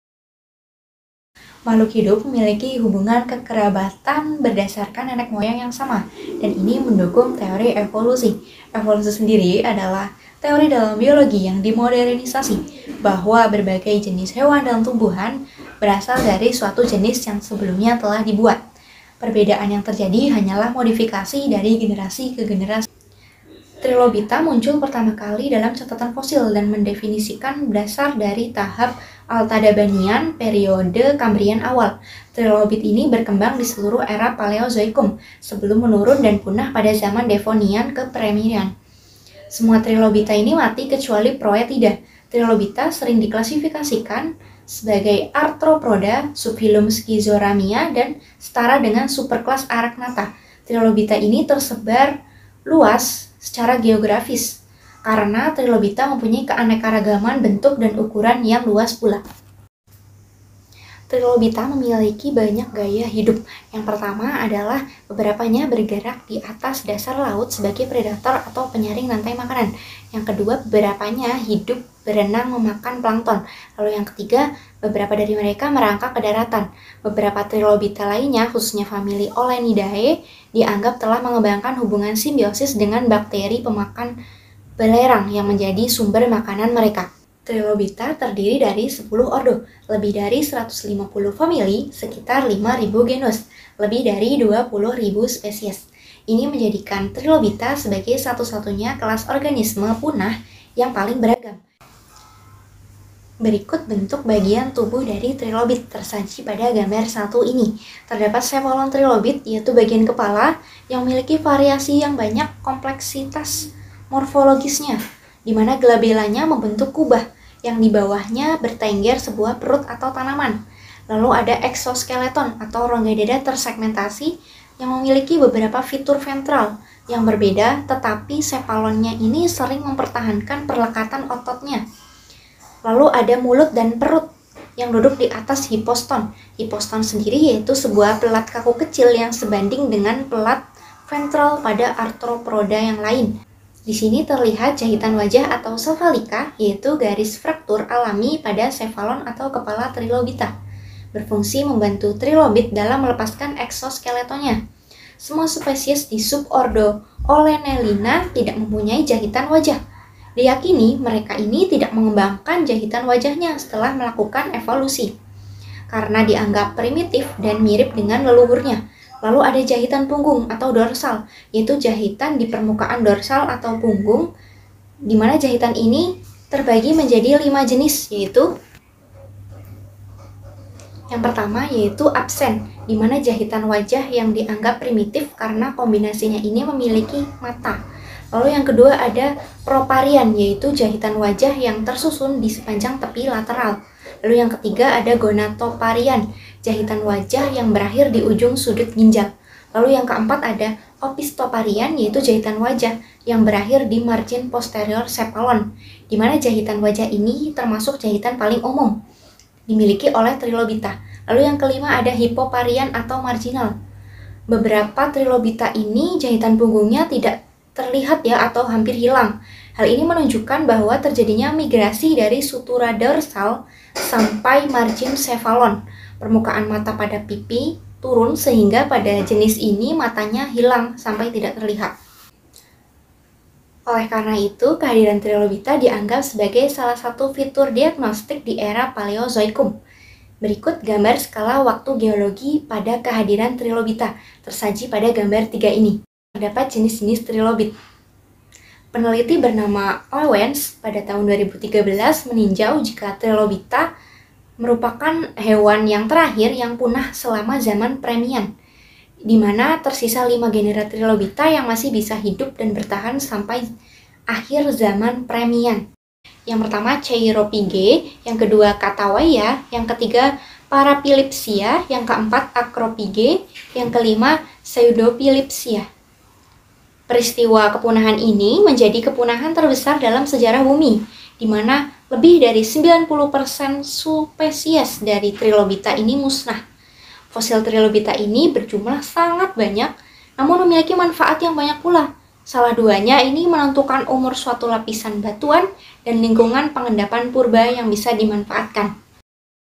EVOLUSI TRILOBITA
Story Telling untuk memenuhi tugas mata kuliah Evolusi
dibuat oleh:
Axioma Salsabila Aniq
Pendidikan Biologi 2019